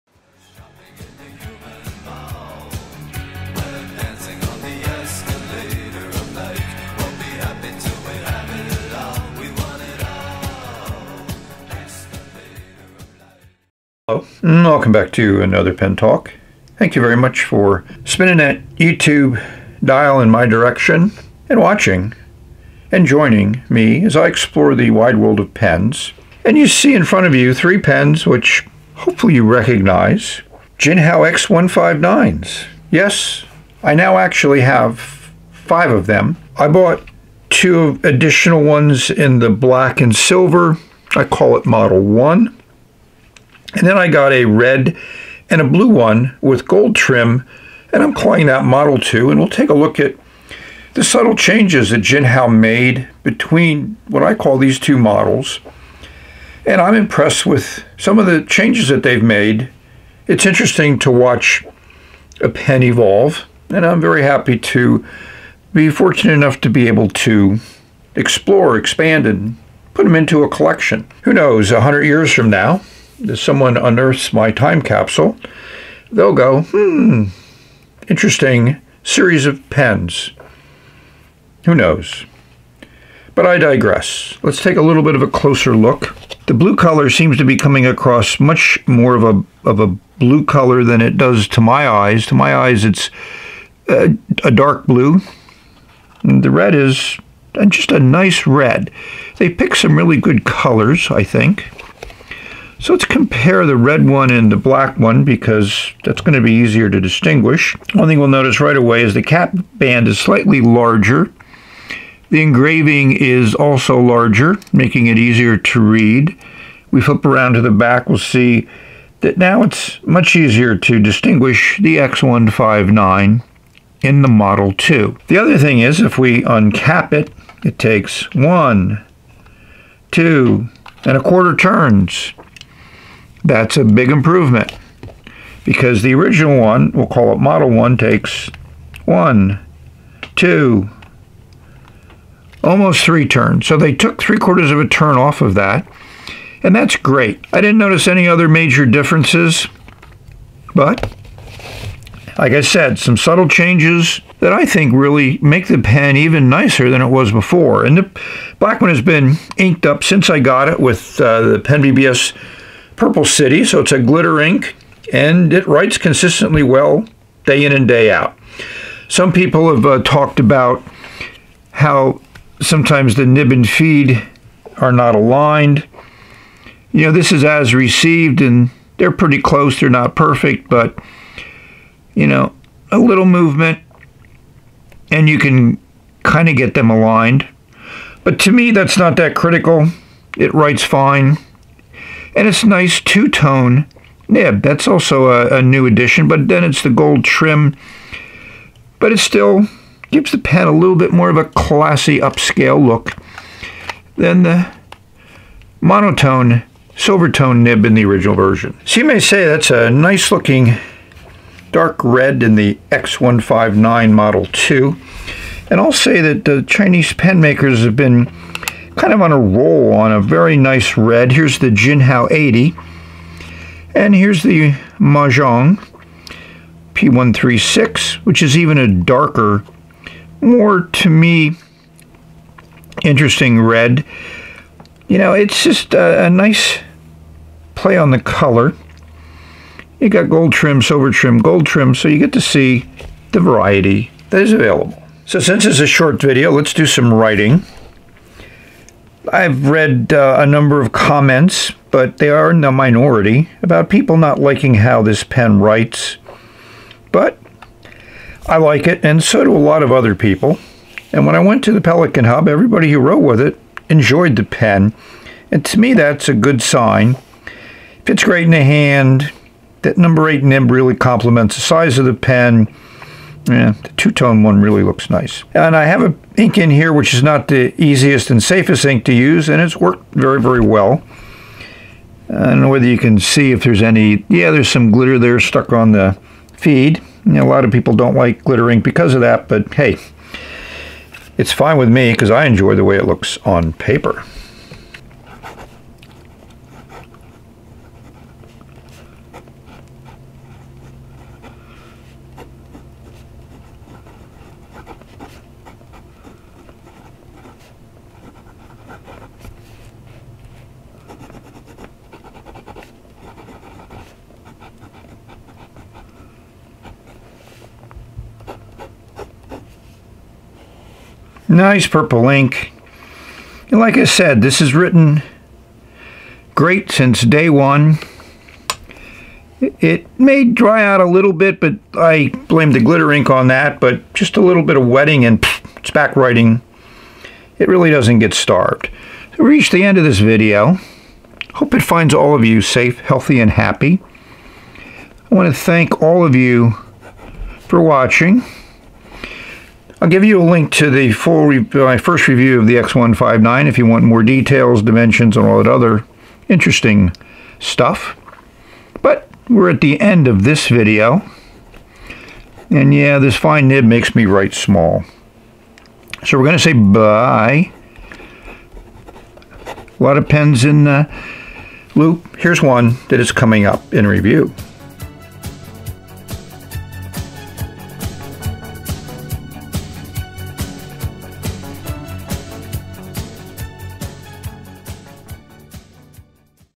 Jinhao X159 Model2 Fountain Pen Review
Jinhao X159 has some minor changes.  I call it Model 2.  See the blue & red with Golden trim.  I like the pen.  I have 5, with 2 more on the way.